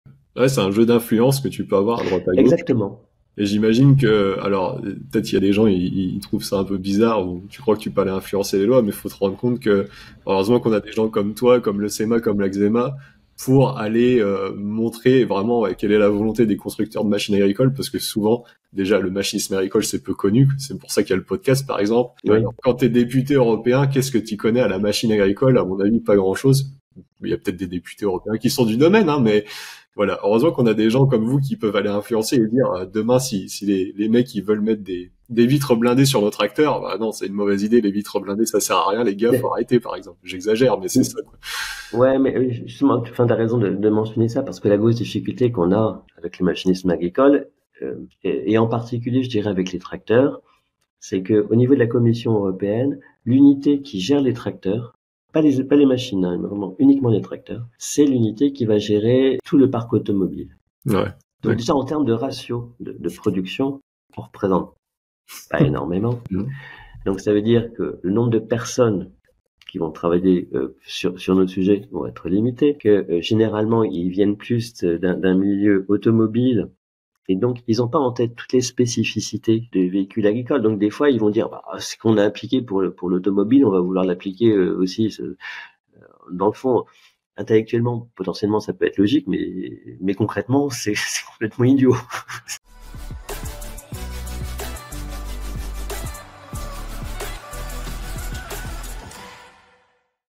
Content d'avoir un syndicat européen pour recadrer les discussions sur les tracteurs
Quand tu vois que l'industrie automobile prend beaucoup de place grâce son volume de production.

Parfois, nos politiques se disent que c'est la même chose dans le matériel agricole : facile !

Heureusement, Christophe et ses collègues du CEMA veillent au grain.
T'as trouvé ce post utile ou sympa ? Mets un like pour me soutenir 😍